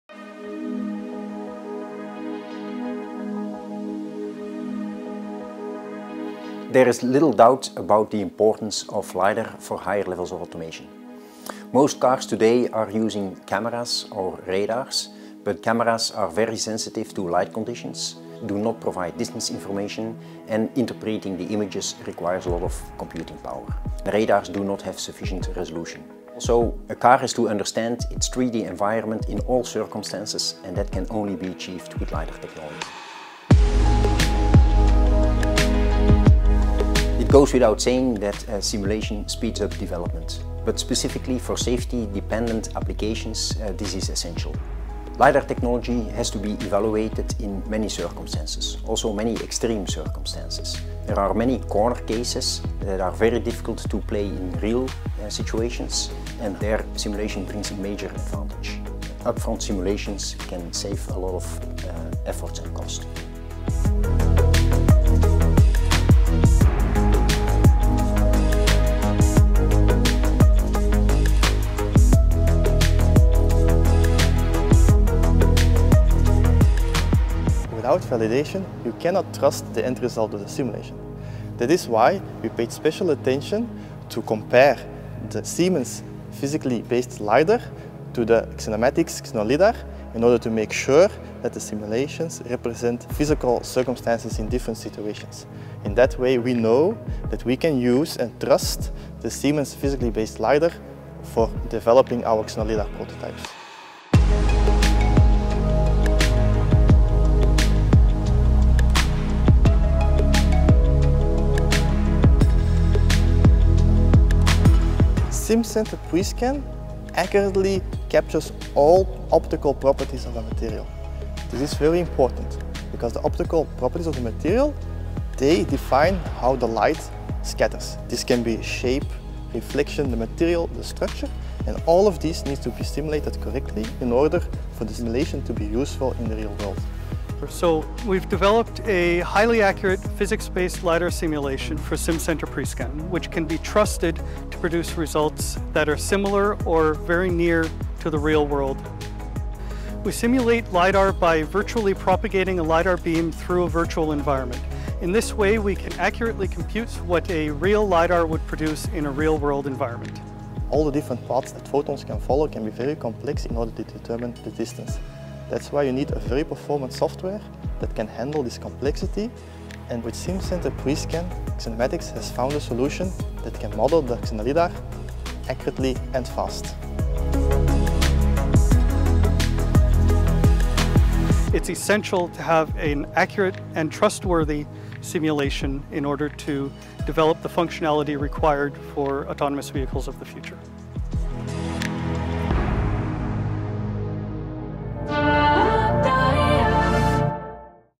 XenomatiX | Physics-based lidar simulation for high levels of automation | Simcenter
When developing higher levels of autonomous driving, the validation of your lidar system becomes more important. As this technology has to be evaluated in extreme conditions, simulation brings major advantages, such as speed, reliability, and accuracy.